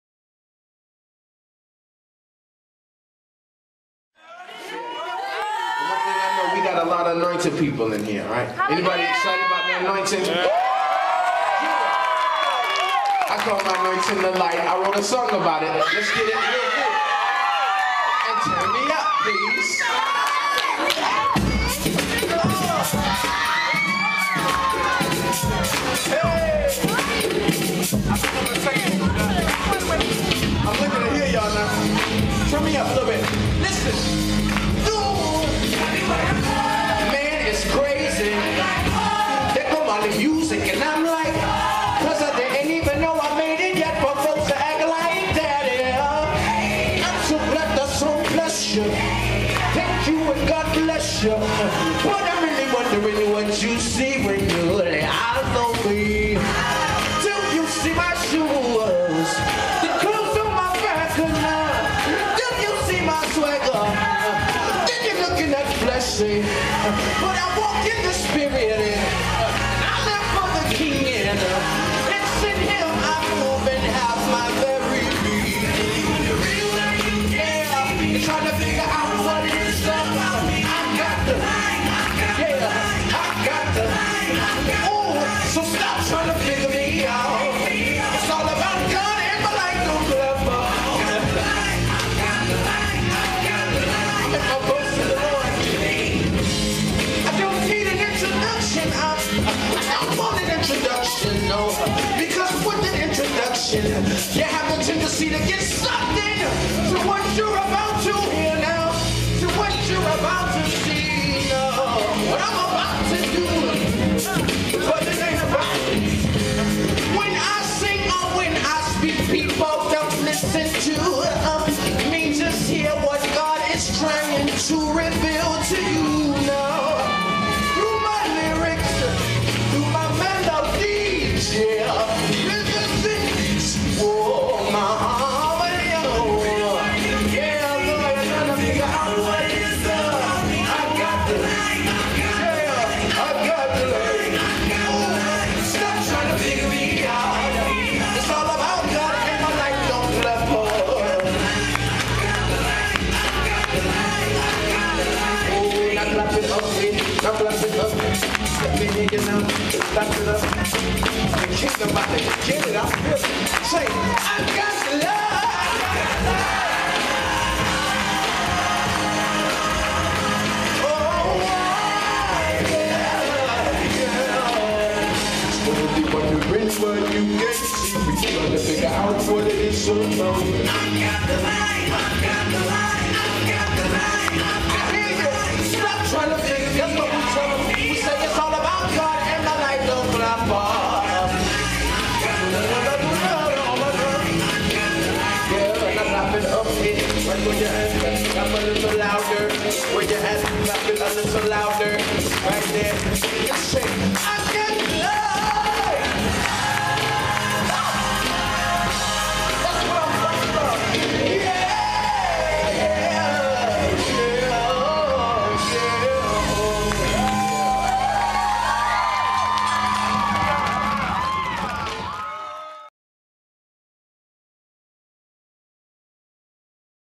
Mali Music & Breathe Life Radio at King Conscious, Pt 3 of 11
were blessed to have been able to attend the King Conscious event at South Sacramento Christian Center on April 3, 2010, getting the chance to experience Mali Music being used by God to minister... the experience was AMAZING!

As much as you will get from watching these vids (and you can see them all it can not convey what it is to be IN the presence of the anointing!

Find out when Mali Music will be in your area and GO... and if he isn't gonna be in your area, contact his management and make it happen!